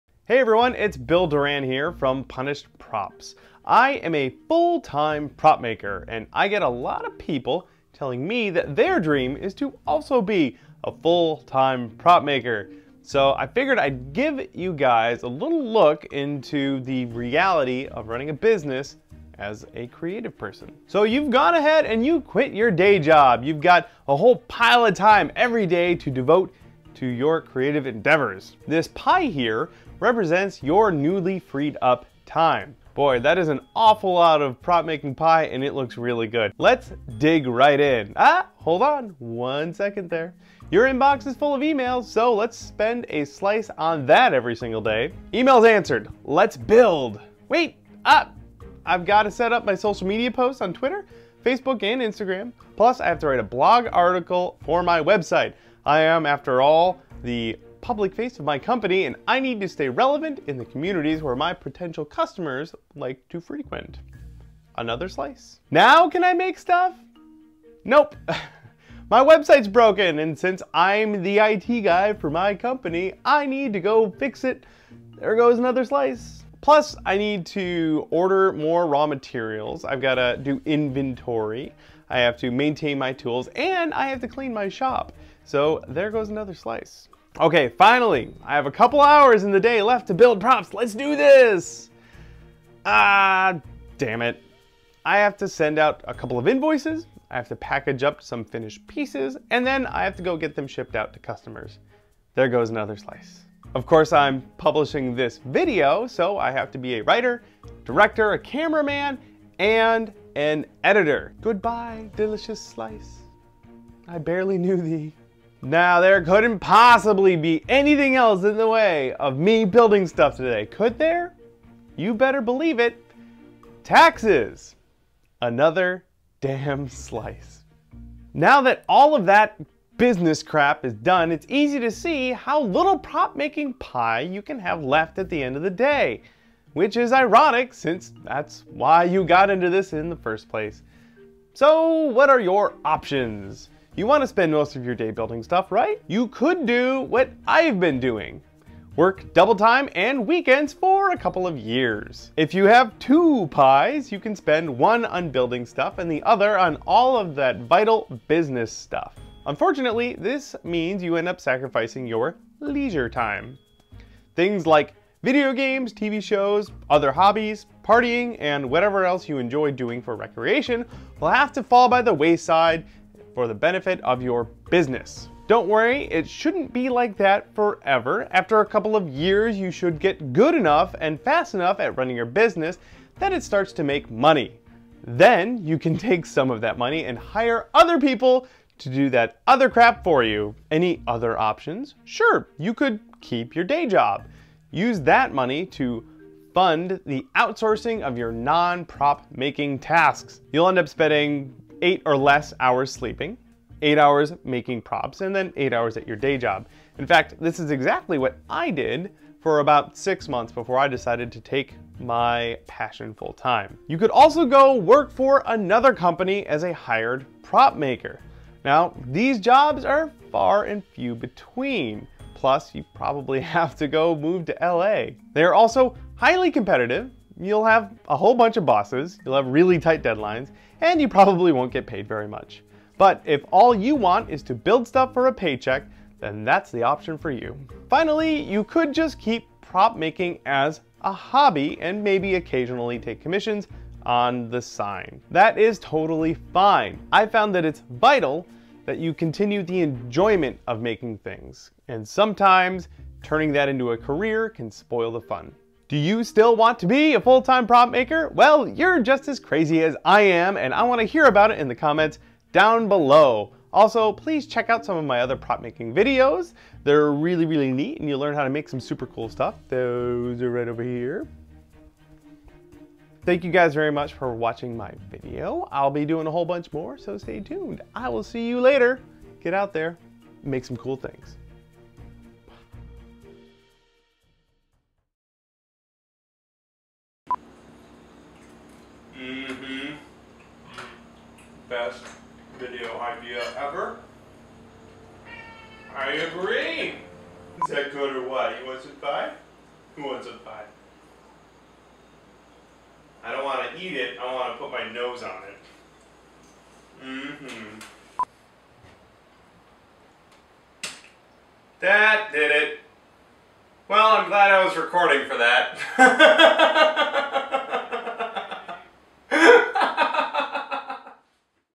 Prop: Talk - The Reality of Running a Creative Business
Is your dream to become a full-time replica prop maker? Bill shares his experience of running a business as a creative person.